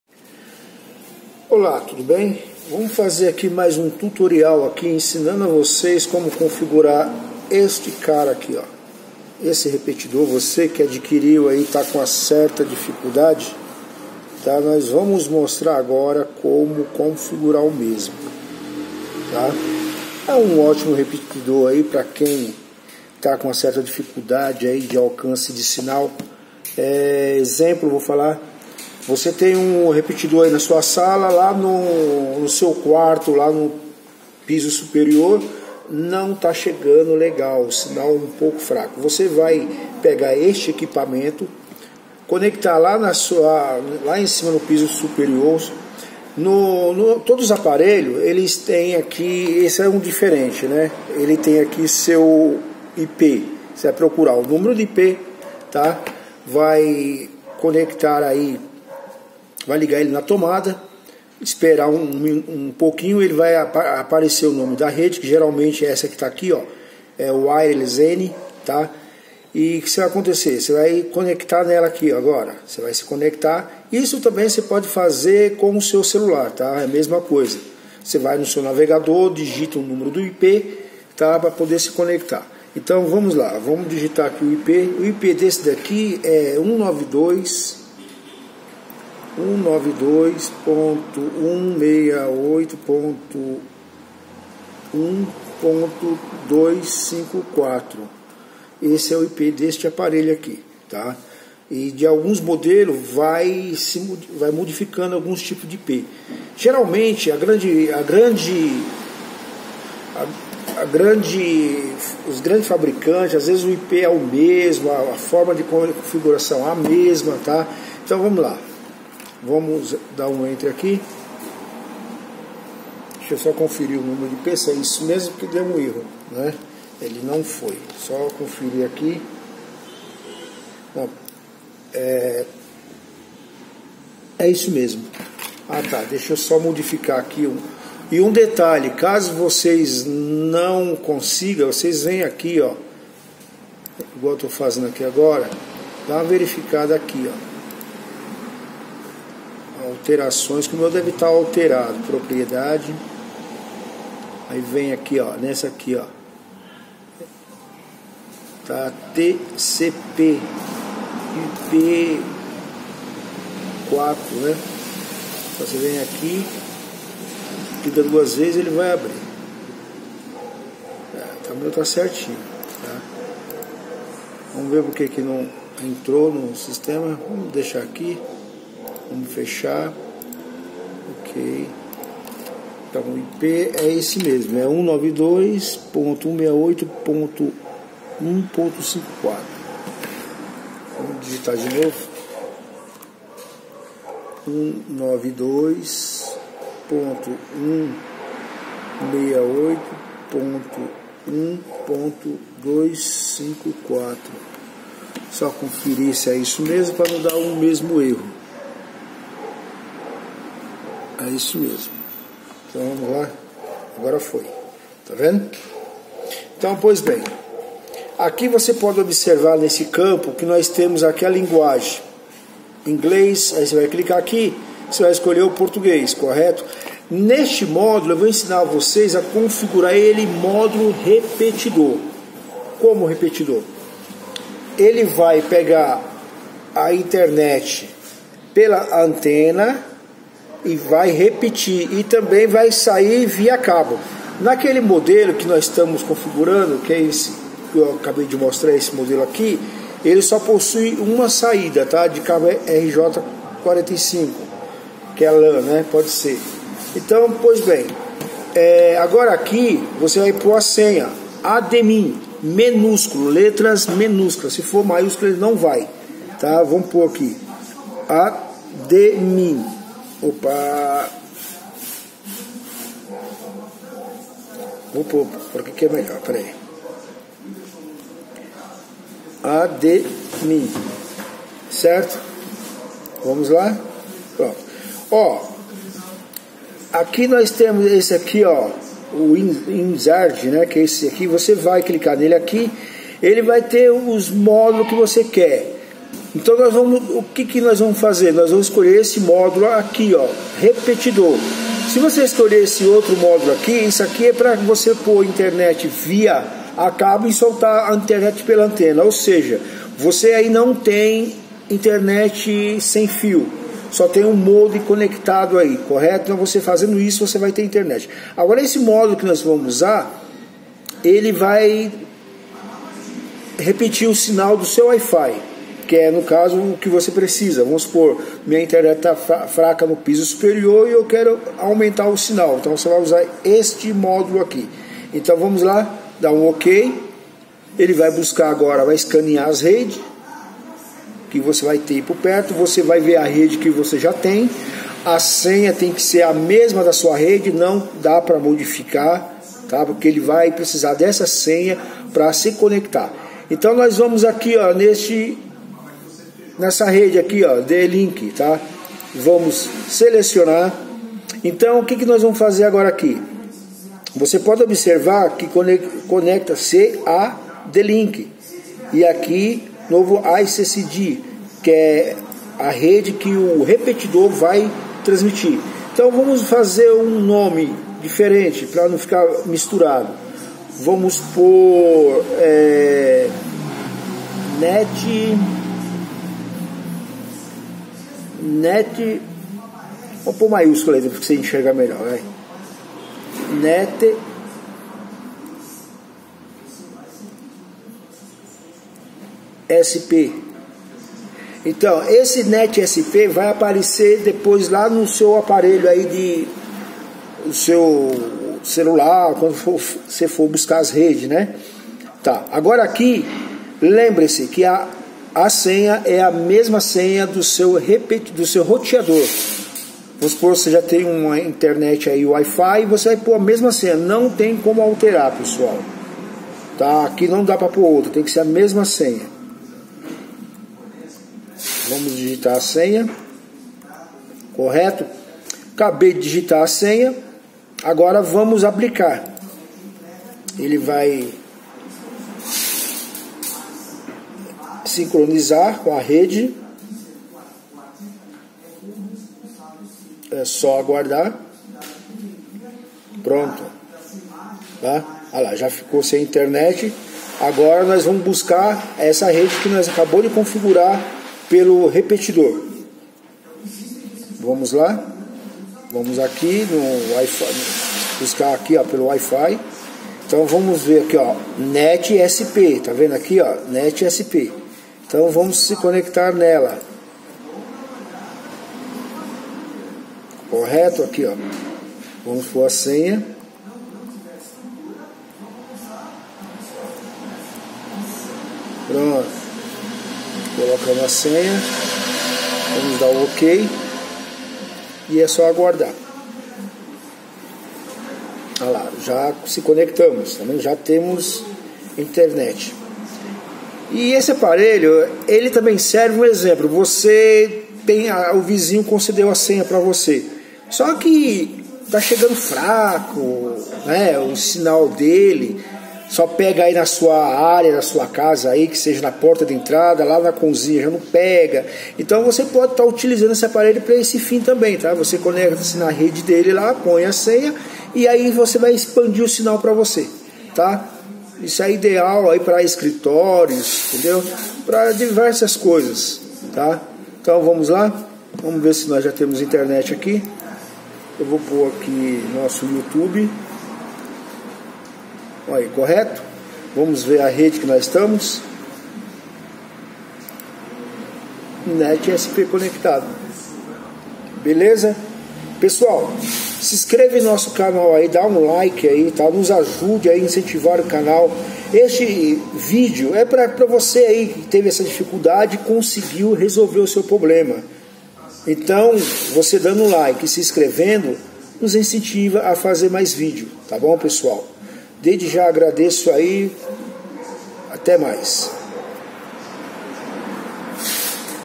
CONFIGURAÇÃO REPETIDOR WIRELESS- N MINI ROUTER - ATUALIZADO 2025
📊 Tutorial completo de configuração de repetidor de Wi-Fi. Desde a escolha do dispositivo até as etapas detalhadas de configuração, este vídeo irá simplificar o processo para garantir uma extensão eficaz do seu sinal sem fio. Confira agora e amplie sua experiência de conexão!

USUÁRIO: admin
SENHA: admin

🚨 IMPORTANTE!

• Antes de configurar, verifique o numero do IP atrás do repetidor, ligue na tomada e se conecte ou pelo cabo ou pelo Wi-Fi no celular.

• Quando for configurar o repetidor de Wi-Fi, a senha tem que ser a mesma do seu modem, não pode ser diferente!
Caso tenha feito isso ele irá ficar sem internet.